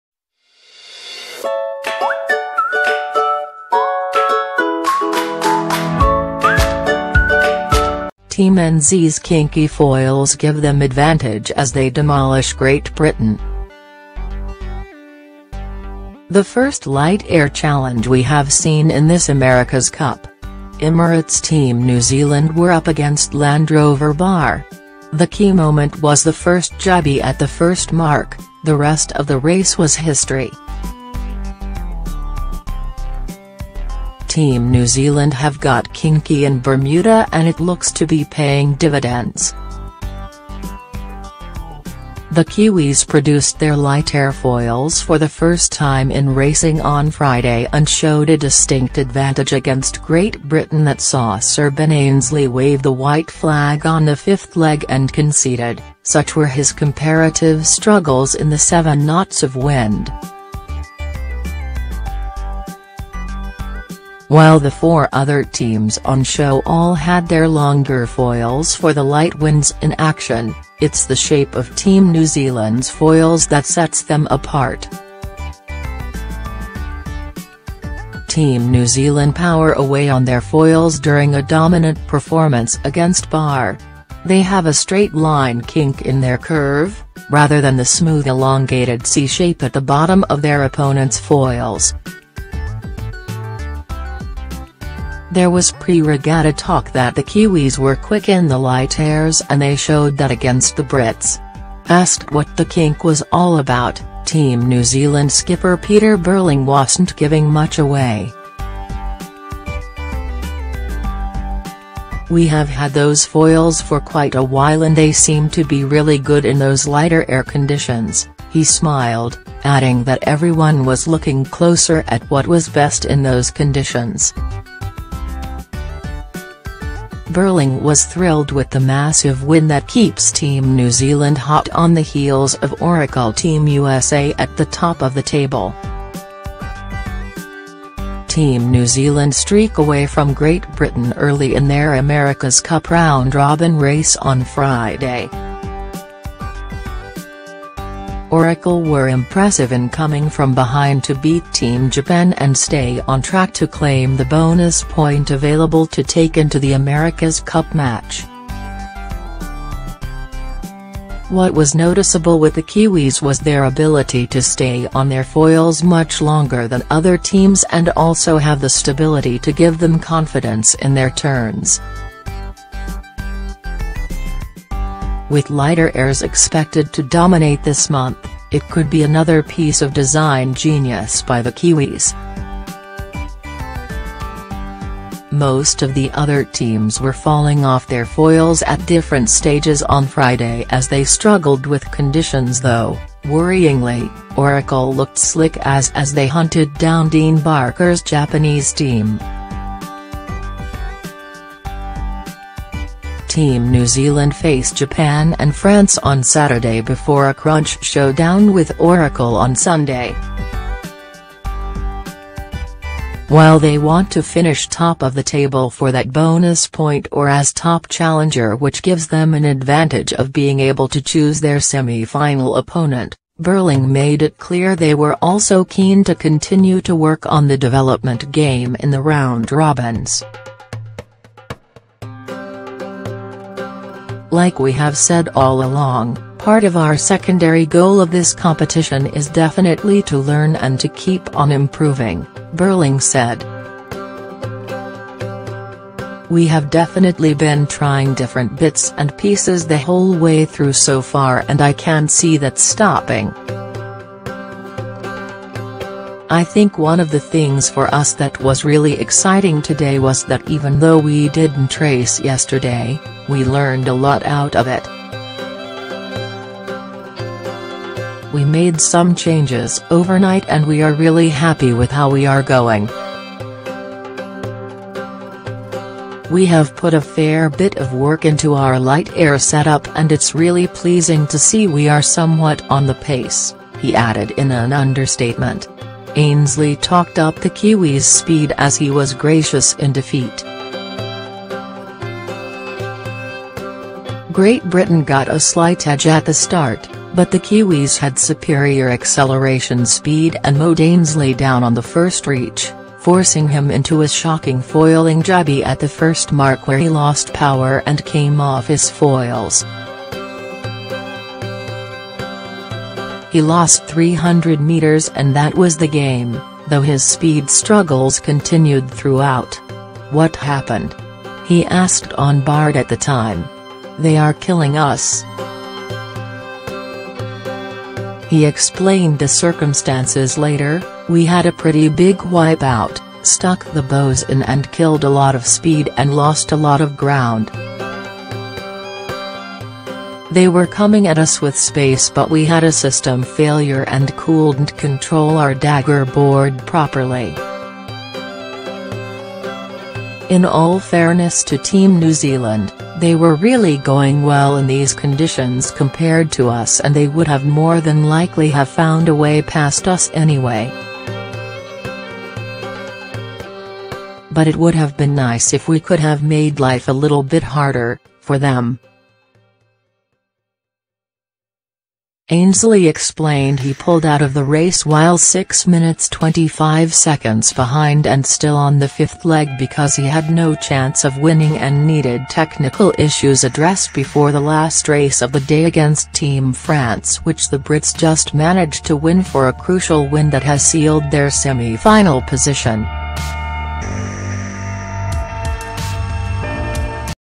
Team NZ's 'kinky' foils give them advantage as they demolish Great Britain
NEWS DAYS 24H: Team NZ's 'kinky' foils give them advantage as they demolish Great Britain